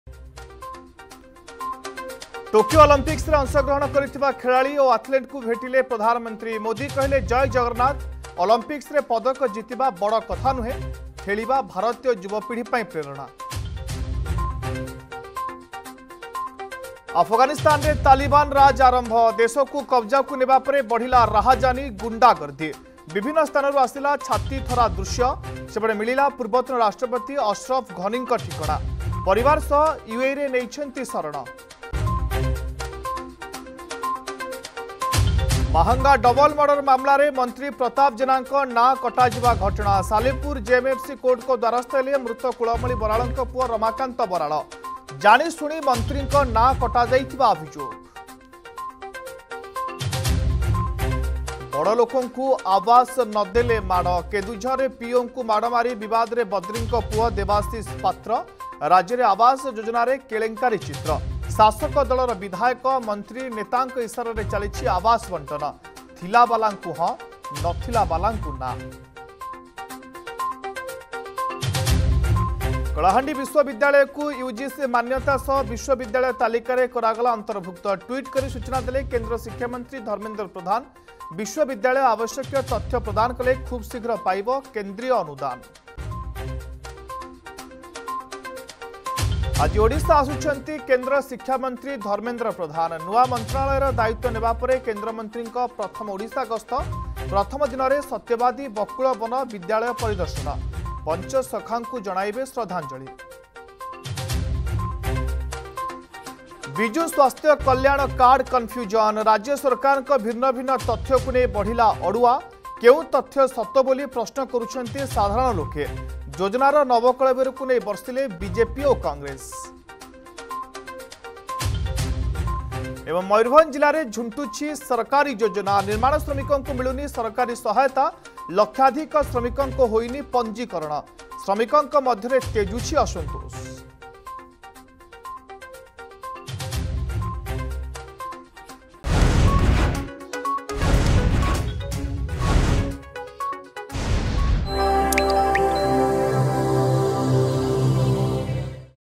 Headlines 7AM | 19th August 2021 | Argus News
Headlines 7AM
+ ଖେଳାଳି ଓ ଆଥଲେଟଙ୍କୁ ଭେଟିଲେ ପ୍ରଧାନମନ୍ତ୍ରୀ
+ ଆଫଗାନିସ୍ତାନରେ ତାଲିବାନ ରାଜ ଆରମ୍ଭ
+ ମନ୍ତ୍ରୀ ପ୍ରତାପ ଜେନାଙ୍କ ନାଁ କଟାଯିବା ଘଟଣା
+ ବଡ ଲୋକଙ୍କୁ ଆବାସ ନ ଦେଲେ ମାଡ
+ କଳାହାଣ୍ଡି ବିଶ୍ବବିଦ୍ୟାଳୟକୁ ୟୁଜିସି ମାନ୍ୟତା ସହ ବିଶ୍ବବିଦ୍ୟାଳୟ ତାଲିକାରେ କରାଗଲା ଅନ୍ତର୍ଭୁକ୍ତ
+ ଆଜି ଓଡିଶା ଆସୁଛନ୍ତି କେନ୍ଦ୍ର ଶିକ୍ଷାମନ୍ତ୍ରୀ ଧର୍ମେନ୍ଦ୍ର ପ୍ରଧାନ
+ ବିଜୁ ସ୍ବାସ୍ଥ୍ୟ କଲ୍ୟାଣ କାର୍ଡ କନଫ୍ୟୁଜନ
+ ମୟୂରଭଞ୍ଜ ଜିଲ୍ଲାରେ ଝୁଣ୍ଟୁଛି ସରକାରୀ ଯୋଜନା